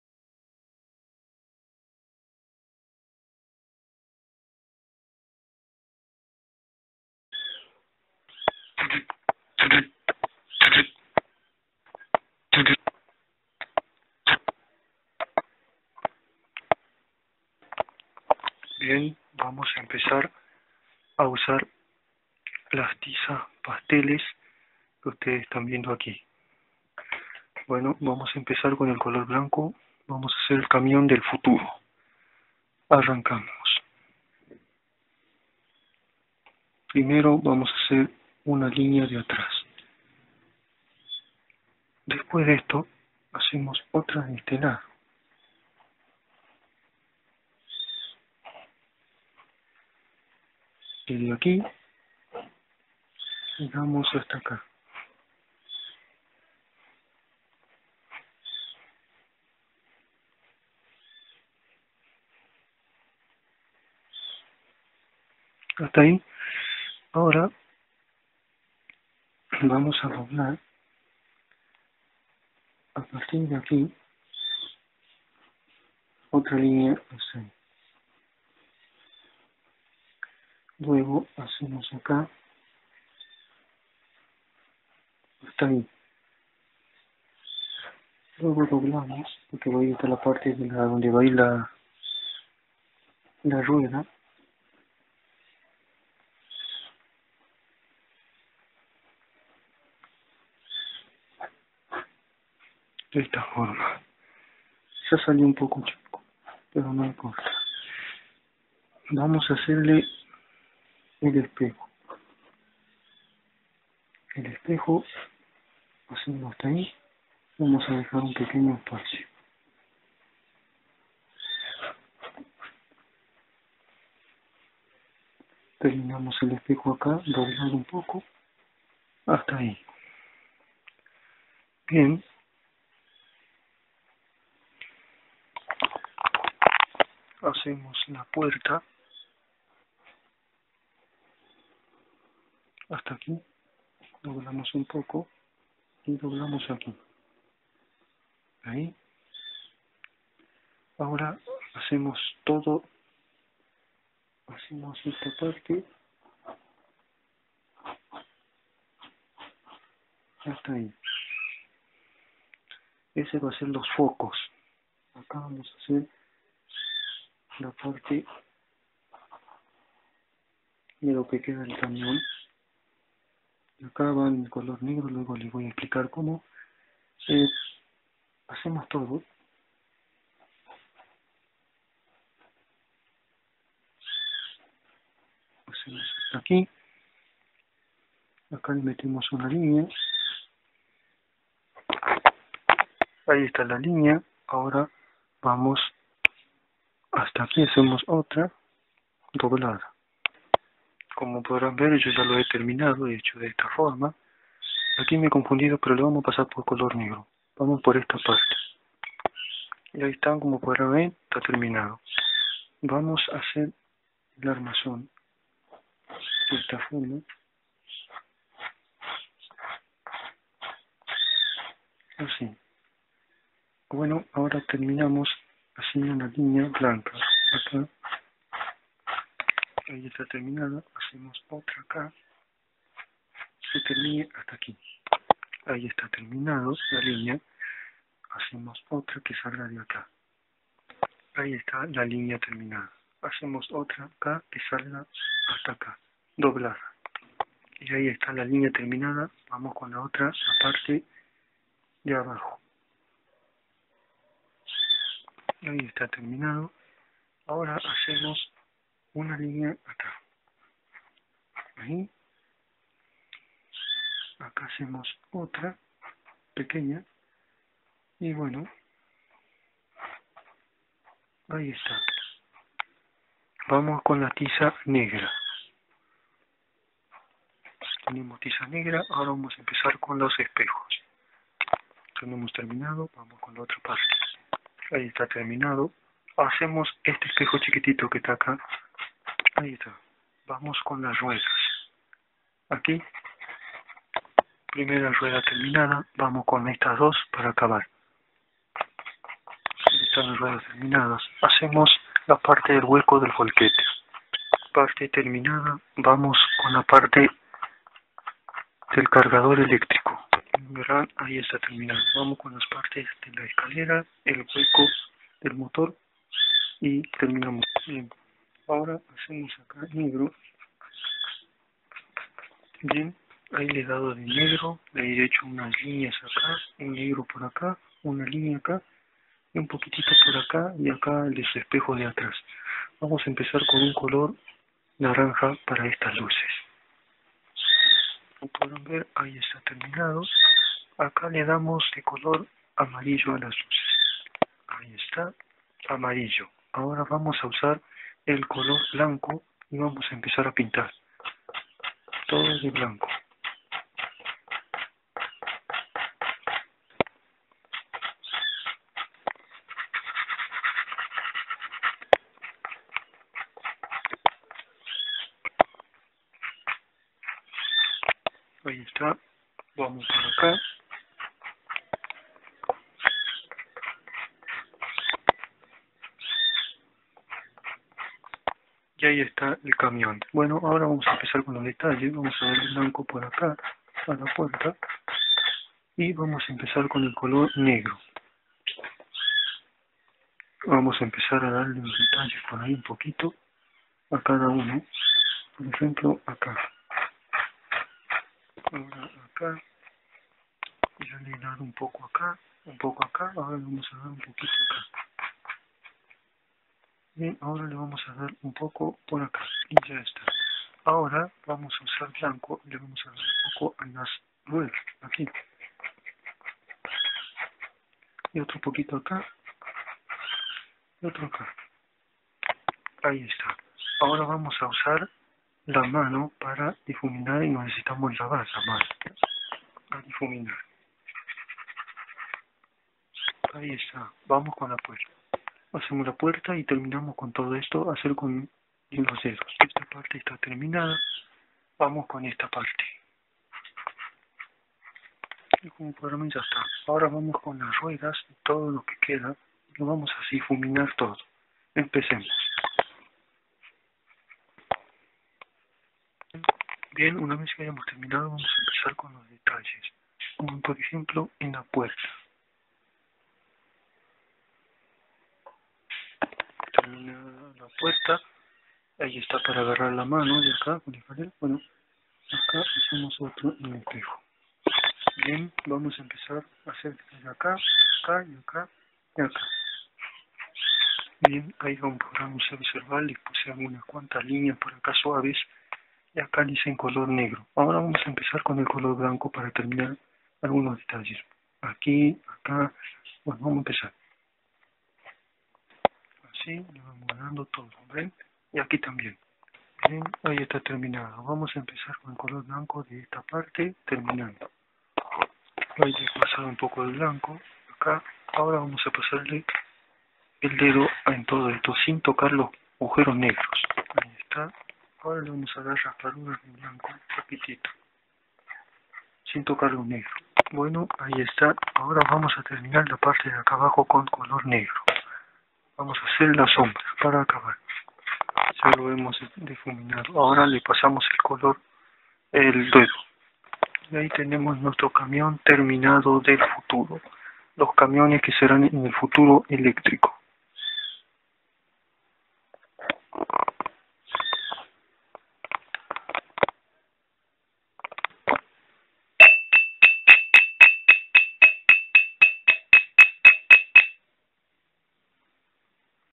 como dibujar un camion del futuro
ASÌ SERAN DENTRO DE UNOS CUANTOS AÑOS LOS CAMIONES EVOLUCIÒN ...el camiòn electrico .